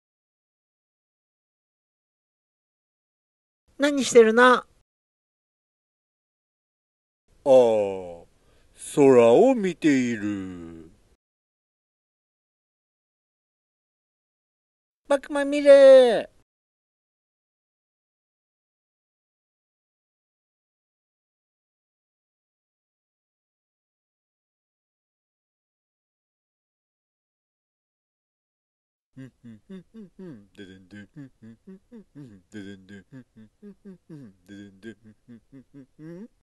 【キャロット＆ブロッコリー】空を見ている【字幕なし】
この世に生を受けたキャロットとブロッコリーの物語です。英語および日本語の学習用教材として作られました。ほんとなんだってば。英語翻訳版： テキストはこちら：